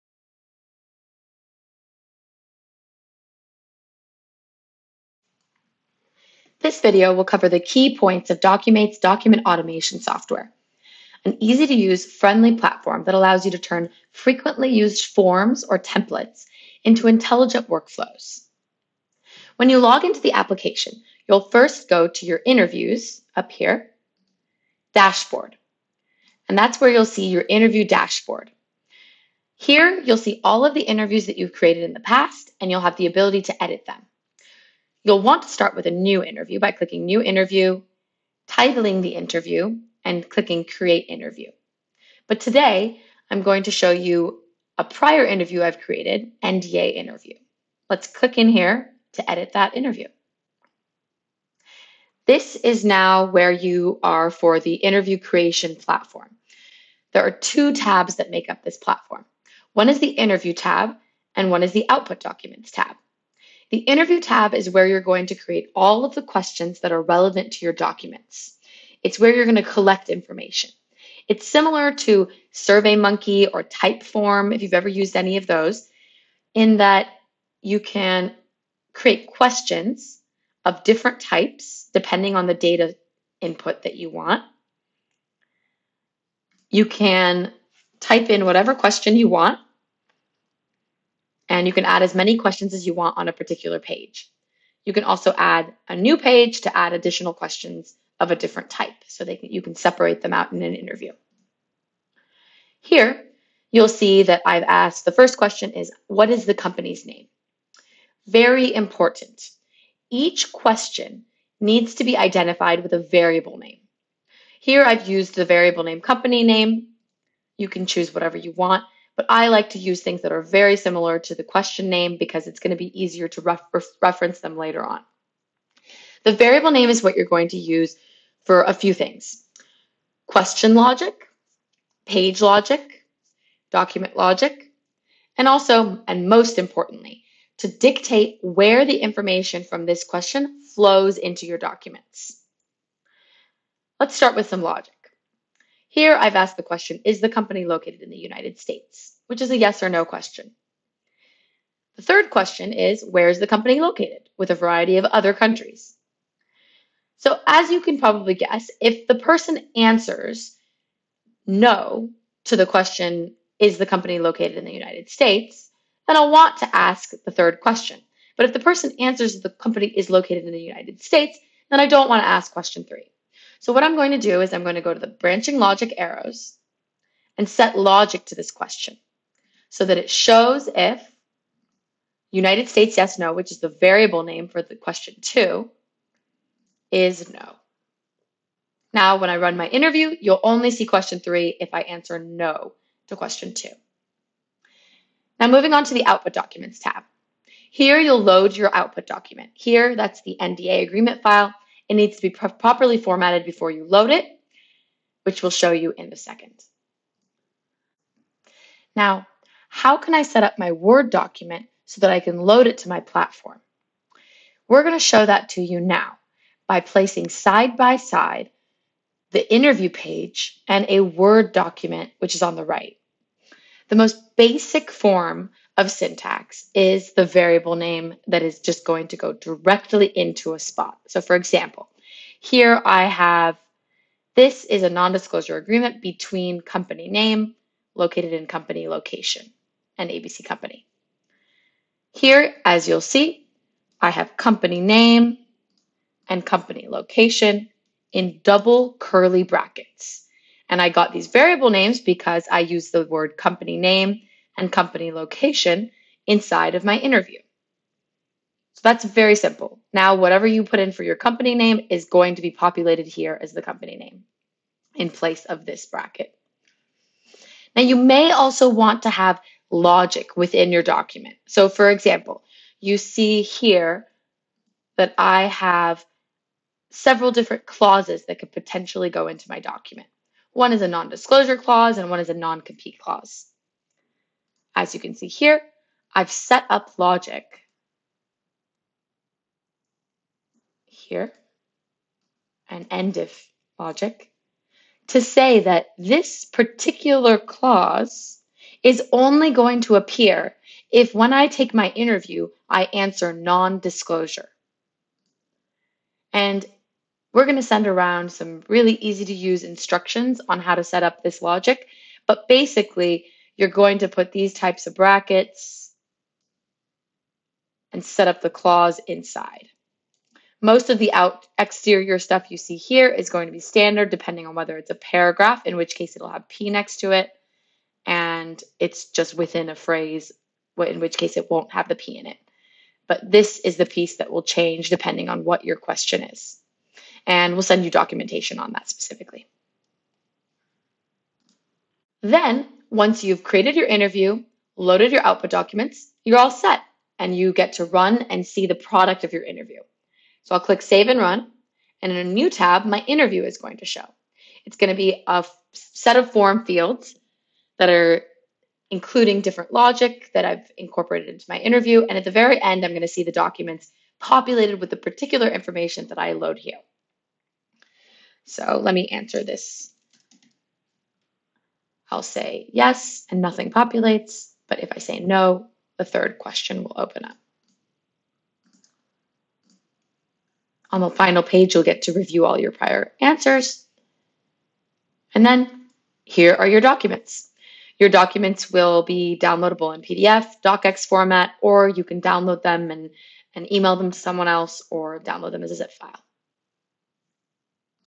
Documate Example: Automating an NDA on Documate
An overview of the document automation and document assembly process with Documate, the no-code document automation software built on Docassemble.  It shows how to automate an NDA in about 5 minutes.